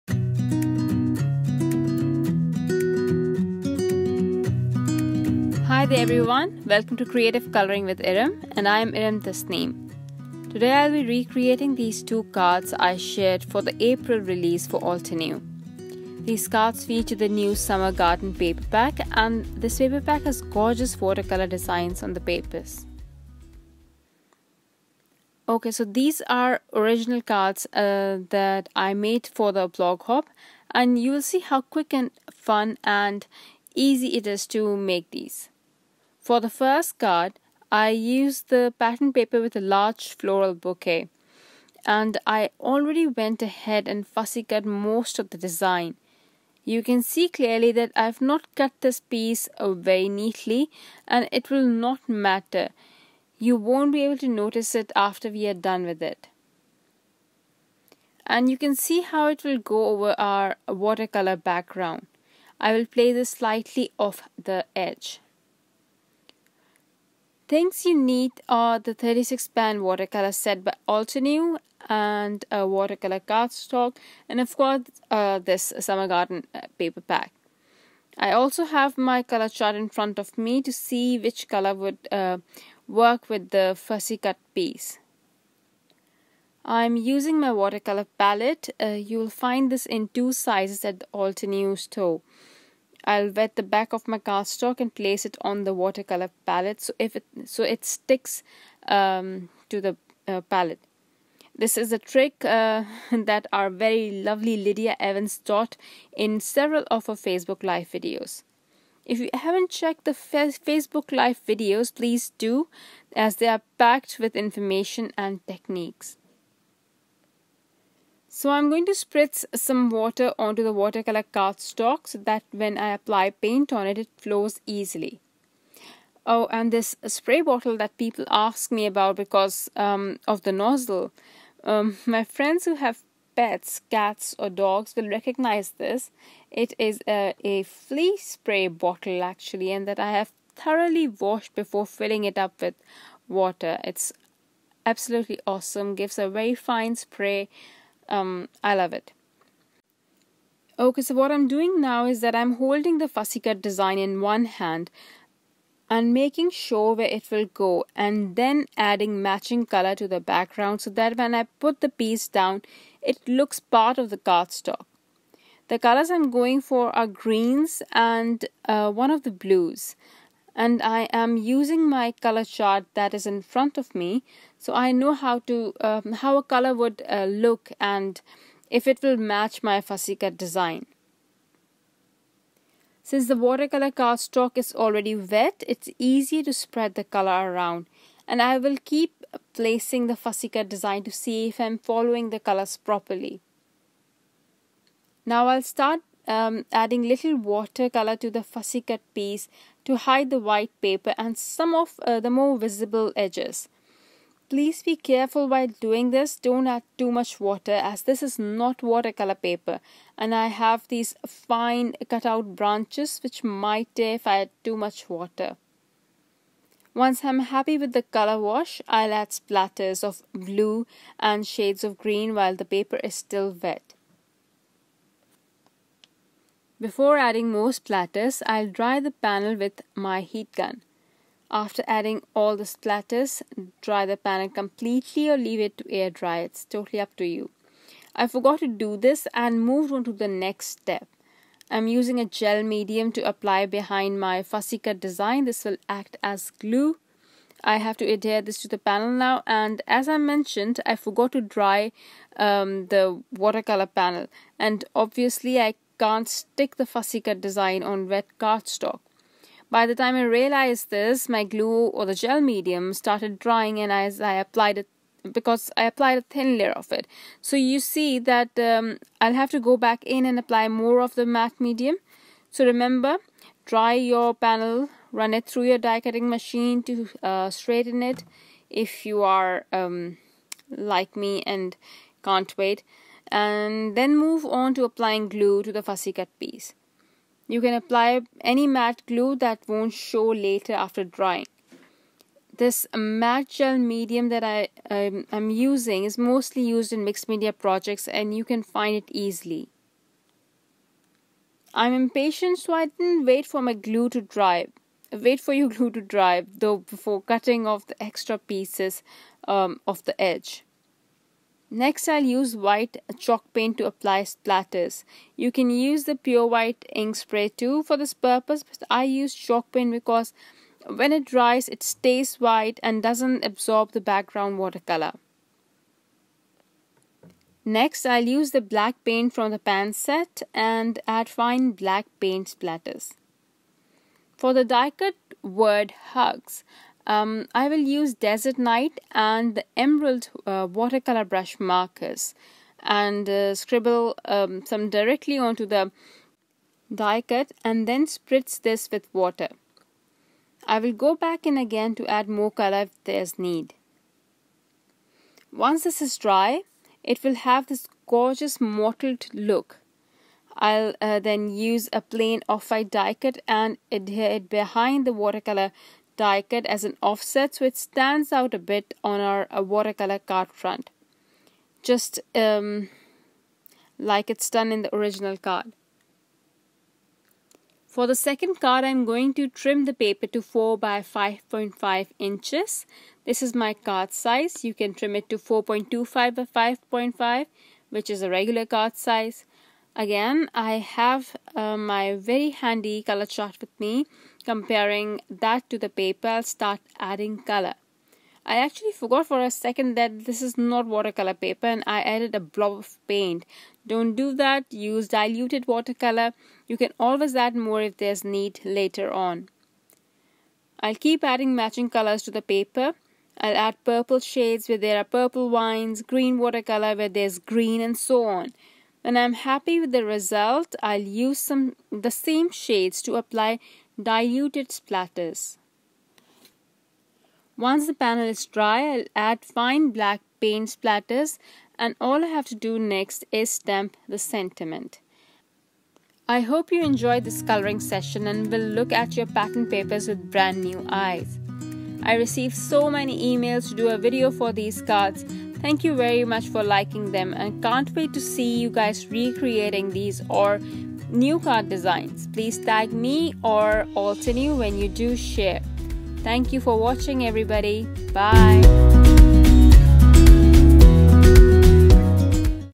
Creative Coloring with Erum: Adding Watercolor Details to Patterned Paper
Hello! Erum Tasneem here with Creative Coloring with Erum. Today I am showing you how to add more interest to your patterned papers by just adding a little watercolor to them and turning them into art pieces. You can check the blog post for more photos and details

to shop for your crafting stash.